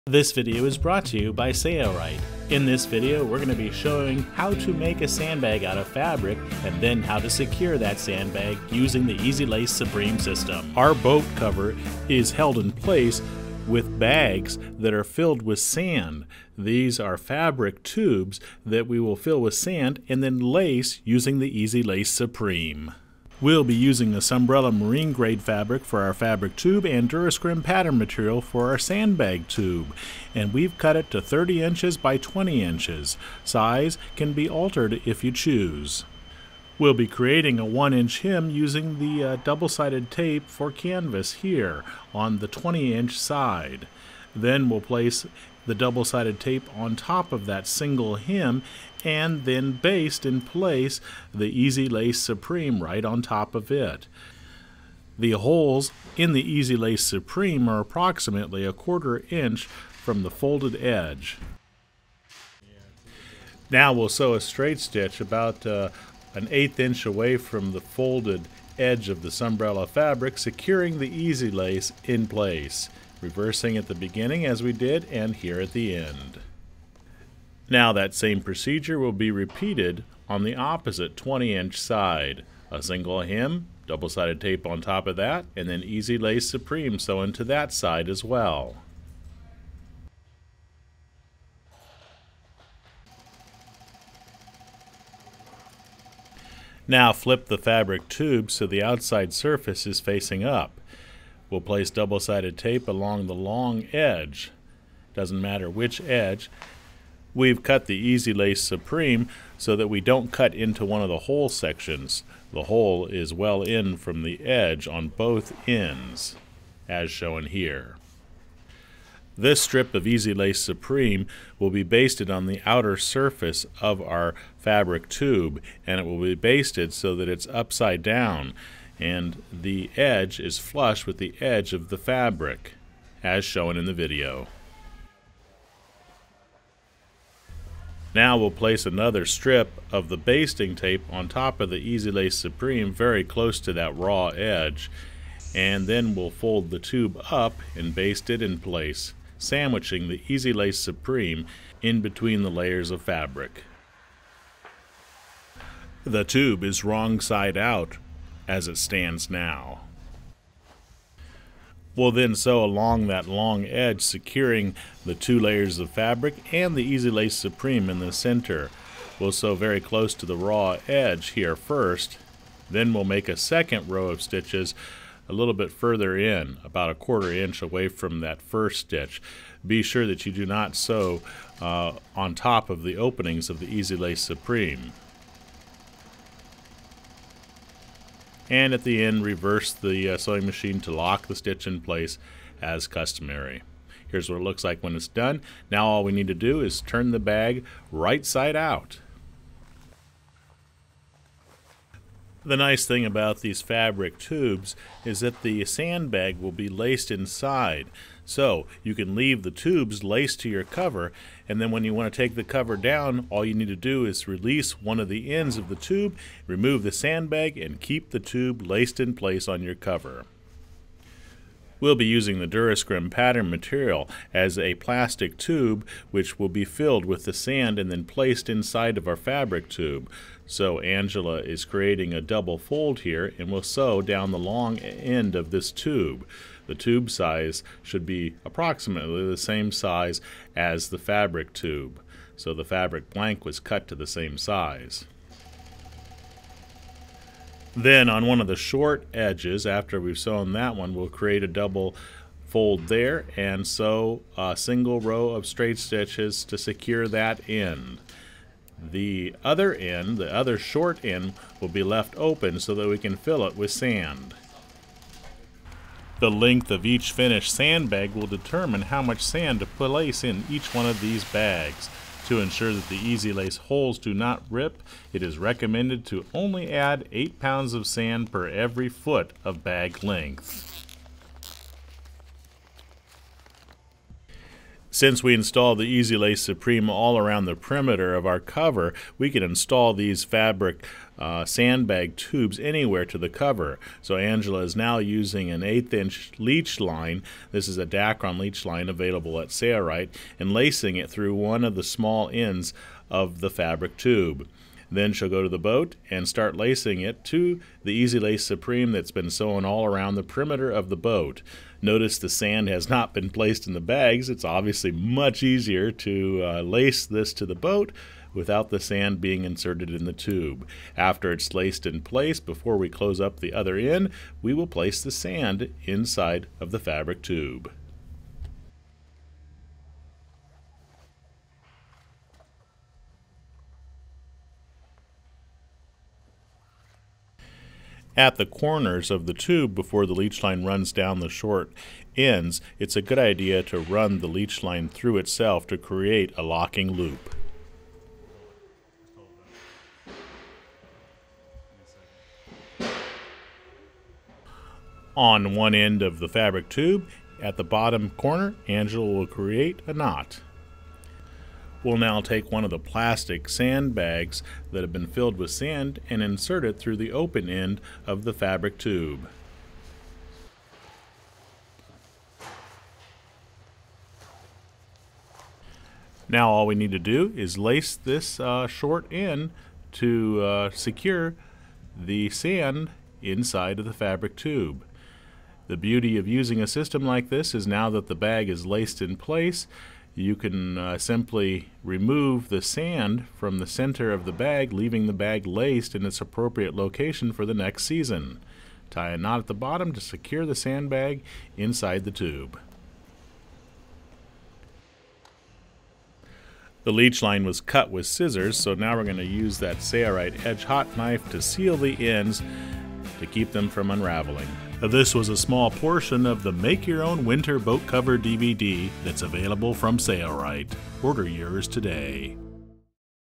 How to Secure a Boat Cover with Sandbags
How to Secure a Boat Cover with Sandbags Video is an excerpt from Sailrite's Make Your Own Winter Boat Cover DVD. This video shows you step-by-step how to make sandbags and fabric tubes to hold the sandbags for a boat cover. In this video we use E-Z Lace Supreme to lace fabric tubes to a boat cover. These sandbags act as weights to hold the cover in place on top of the boat. The system outlined in this video allows the fabric tubes to remain on the cover all year and the sandbags to be removed for storage.

For complete instructions on how to make a boat cover from start to finish get the Make Your Own Winter Boat Cover DVD exclusively from Sailrite